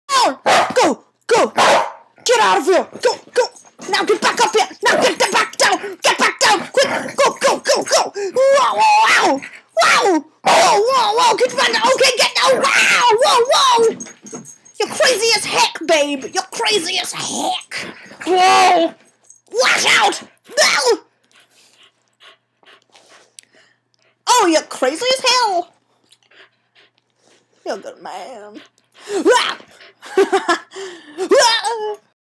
Crazy As Heck!
My dog Leper is often quite calm and well mannered. but sometimes this happens. NO that is not my room.